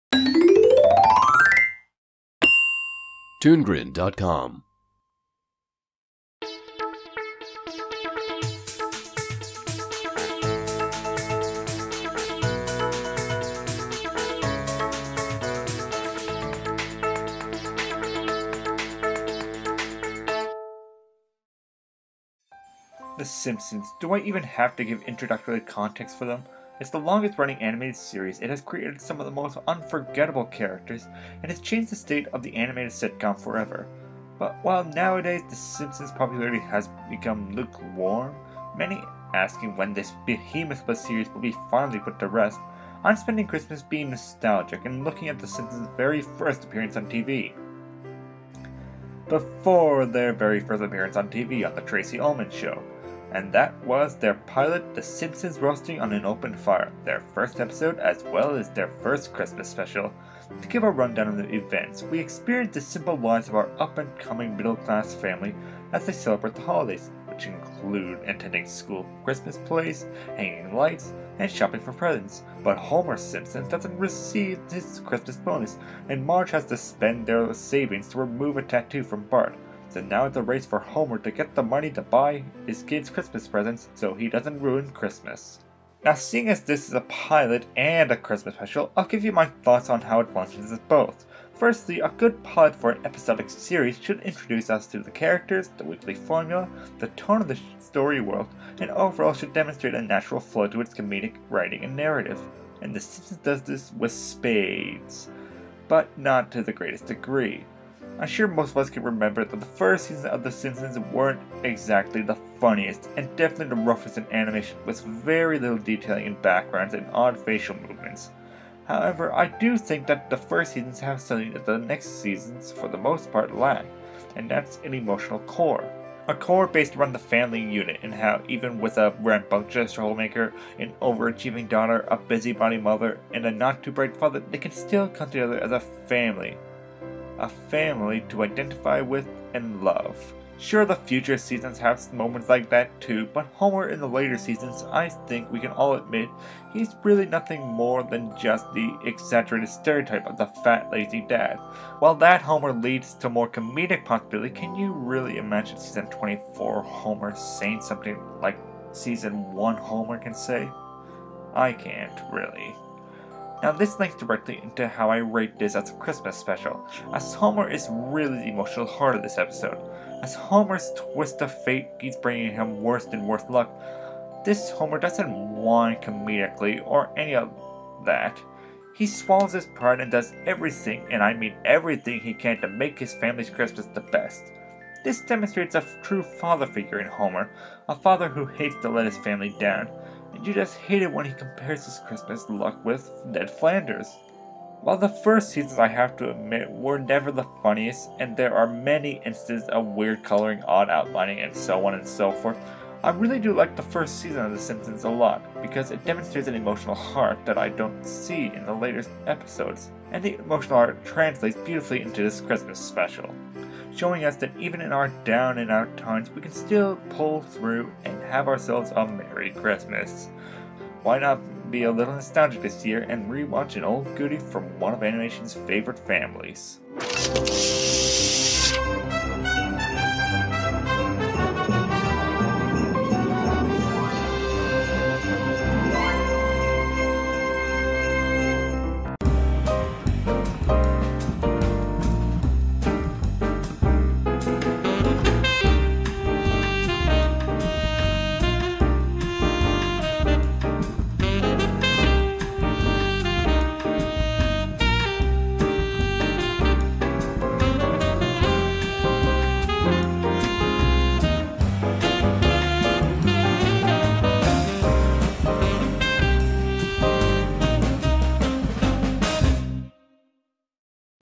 The Simpsons Roasting on an Open Fire Review | Whyboy Spotlights
Whyboy goes to the past this Christmas by rewatching the Simpsons pilot which so happens to be their first Christmas Special as well. ENJOY

FOLLOW WHYBOY ON...

FOLLOW NERO ON...

IN ASSOCIATION WITH...

MUSIC & SOUND EFFECT ASSET PROVIDERS…